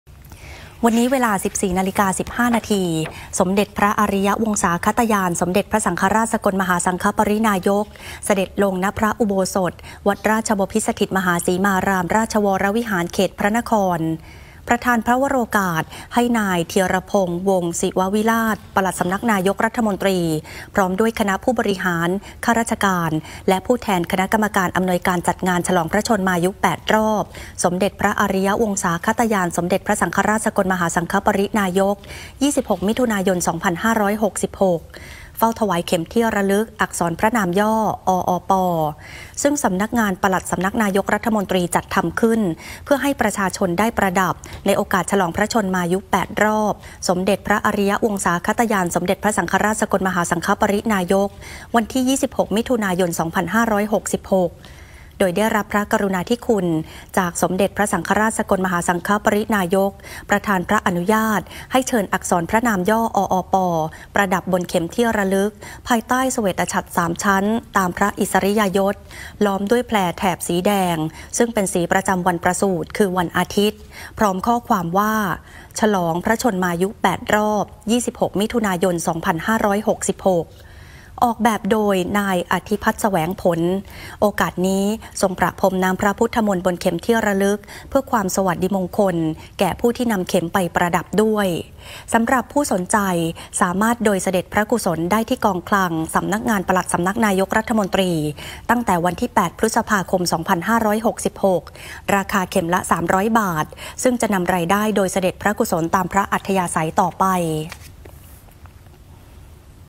สมเด็จพระสังฆราช ประทานพระวโรกาสให้ ปลัดสำนักนายกรัฐมนตรี เฝ้าถวายเข็มที่ระลึกฉลองพระชนมายุ 8 รอบ
วันนี้เวลา 14.15 น. สมเด็จพระอริยวงศาคตญาณ สมเด็จพระสังฆราช สกลมหาสังฆปริณายก เสด็จลง ณ พระอุโบสถ วัดราชบพิธสถิตมหาสีมาราม ประทานพระวโรกาสให้ นายธีระพงษ์ วงศ์ศิวะวิลาส ปลัดสำนักนายกรัฐมนตรี พร้อมผู้แทนคณะกรรมการอำนวยการ จัดงานฉลองพระชนมายุ 8 รอบ สมเด็จพระอริยวงศาคตญาณ สมเด็จพระสังฆราช สกลมหาสังฆปริณายก 26 มิถุนายน 2566 เฝ้าถวายเข็มที่ระลึกฉลองพระชนมายุ 8 รอบฯ ซึ่งคณะกรรมการอำนวยการจัดงานฯ มอบหมายให้สำนักงานปลัด สำนักนายกรัฐมนตรี จัดทำเข็มที่ระลึกฯ เพื่อจำหน่ายสำหรับประชาชนได้ประดับเป็นที่ระลึก และเพื่อนำปัจจัยถวายโดยเสด็จพระกุศลตามพระอัธยาศัย โอกาสนี้ ทรงประพรมน้ำพระพุทธมนต์บนเข็มที่ระลึกฯ เพื่อเป็นสวัสดิมงคล แก่ผู้โดยเสด็จพระกุศลบริจาค เพื่อรับเข็มที่ระลึกฯ ไปประดับ

เข็มที่ระลึกฯ ดังกล่าว ออกแบบเป็นอักษรพระนาม ออป ของสมเด็จพระอริยวงศาคตญาณ สมเด็จพระสังฆราช สกลมหาสังฆปริณายก ภายใต้เศวตฉัตร 3 ชั้น ตามพระอิสริยยศ แวดล้อมด้วยแพรแถบสีแดง อันเป็นสีประจำวันประสูติ คือ วันอาทิตย์ บรรจุข้อความ “ฉลองพระชนมายุ 8 รอบ 26 มิถุนายน 2566”

ผู้สนใจสามารถโดยเสด็จพระกุศล ในราคาเข็มละ 300 บาท ติดต่อที่กองคลัง สำนักงานปลัดสำนักนายกรัฐมนตรี และห้างสรรพสินค้าบิ๊กซี สอบถามรายละเอียดได้ที่ บิ๊กซีคอลเซนเตอร์ โทรศัพท์ 1756 ตั้งแต่วันที่ 8 พฤษภาคมนี้

ข่าวในพระราชสำนักประจำวันที่ 12 เมษายน 2566